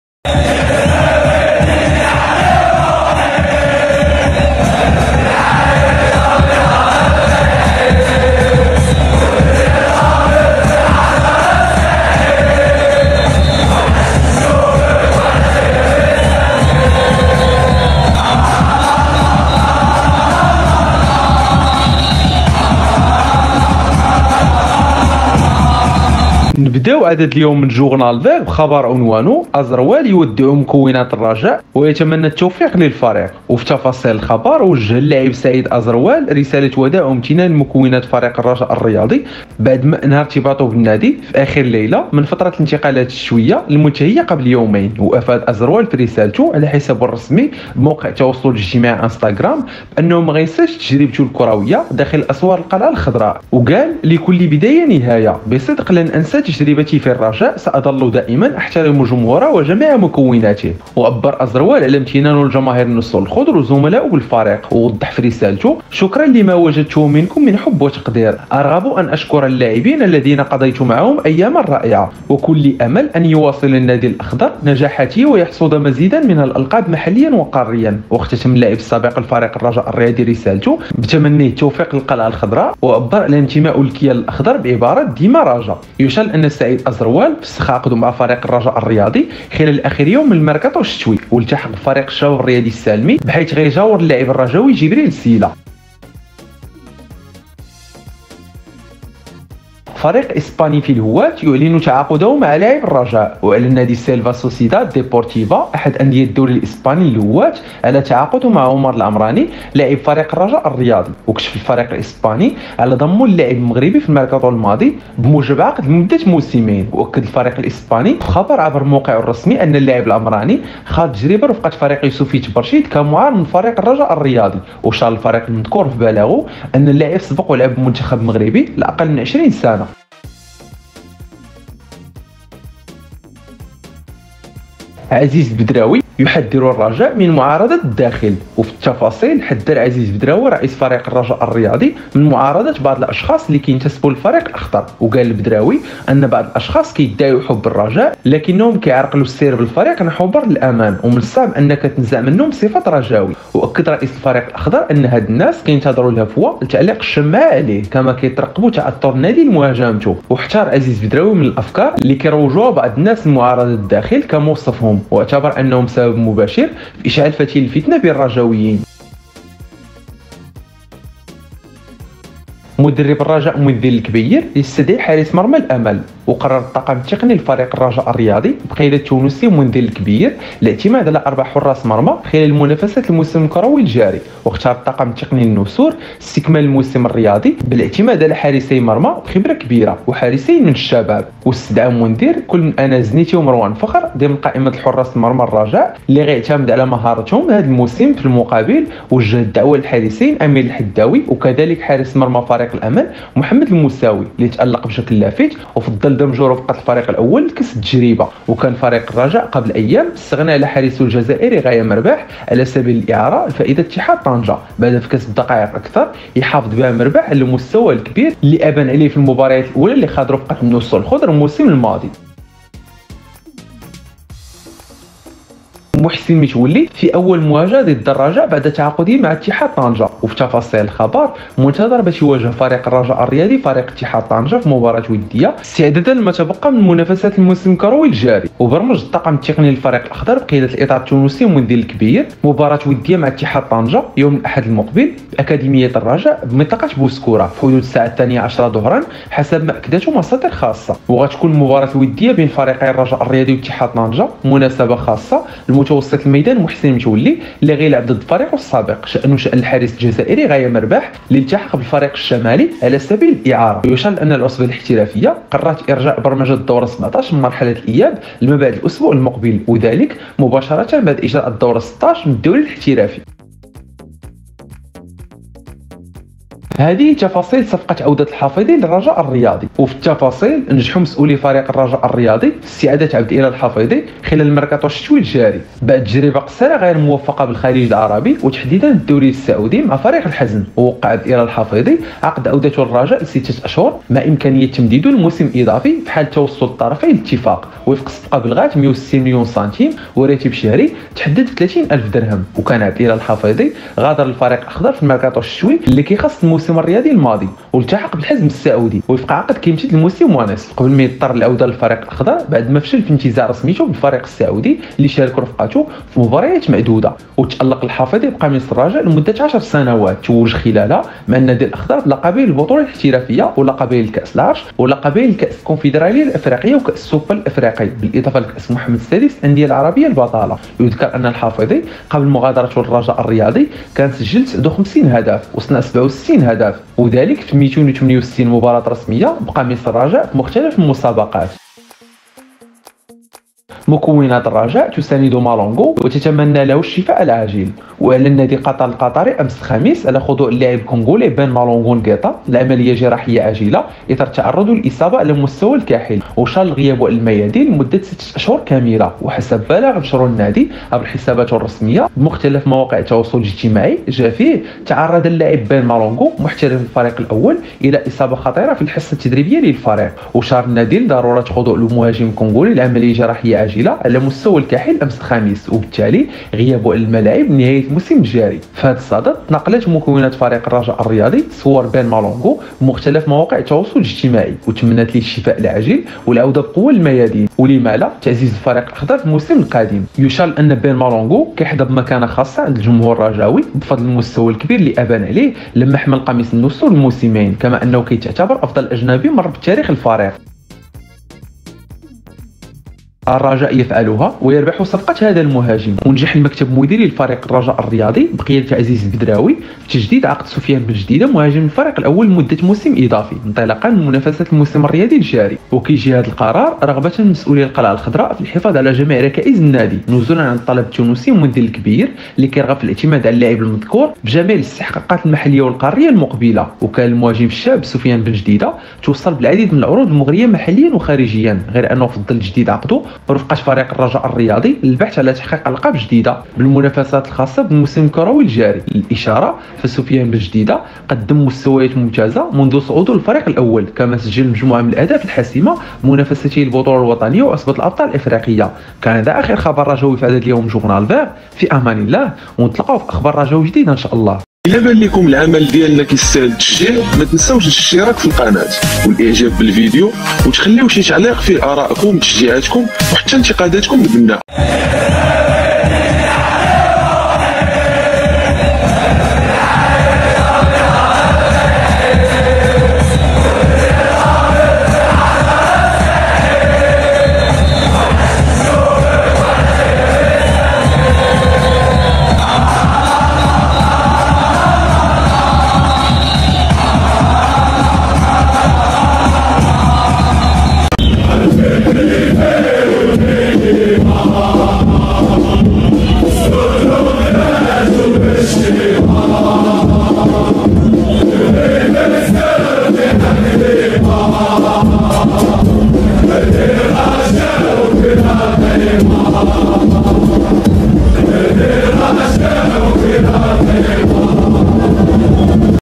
الرجاء يربح هذه الصفقة | التفاتة تجاه مالانغو | متولي يواجه الرجاء | لاعب الرجاء و فريق اسباني
آخر أخبار الرجاء الرياضي لهذا اليوم جاءت عناوينها على الشكل التالي :

ـ سعيد أزروال يودع مكونات الرجاء الرياضي ـ
ـ فريق إسباني في الهواة يعلن تعاقده مع لاعب الرجاء ـ
ـ البدراوي يحذر الرجاء من معارضة الداخل ـ
ـ مدرب الرجاء يستدعي حارس مرمى الأمل ـ
ـ متولي في أول مواجهة ضد الرجاء بعد تعاقده مع ا.طنجة ـ
ـ هذه تفاصيل صفقة عودة الحافيظي للرجاء ـ
ـ مكونات الرجاء تساند مالانغو و تتمنى له الشفاء العاجل ـ
ـ الرجاء يفعلها و يربح صفقة هذا المهاجم ـ